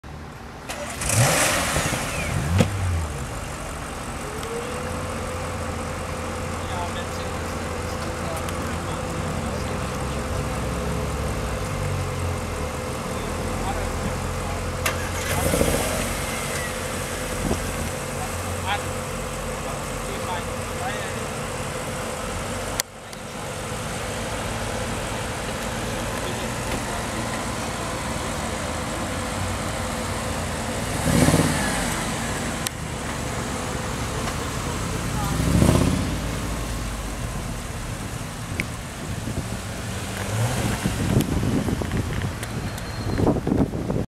[Combo] New Audi RS4 Avant & RS6 Avant @ Nurburgring [Autogespot - Carspotting] 720p HD
New Audi RS4 & RS6 Combo @ Nurburgring [Autogespot - Carspotting] 720p HD

Awesome combo spotted on the parkplatz for the entrance of the nordschleife !